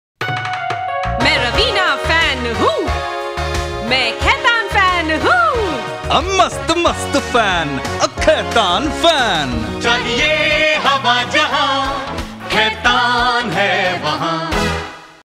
Khaitan Fan - Raveena
Director / Producer - Pooh / Siraj Ayesha Sayani
Year - 1994
Product - Khaitan Fans
Production House - Big Mama
Agency - HTA
DOP -
Actors - Anu Koitoor
Assi. Director & Post Production – Madhukar Gaikar
VO - Chetan Shashital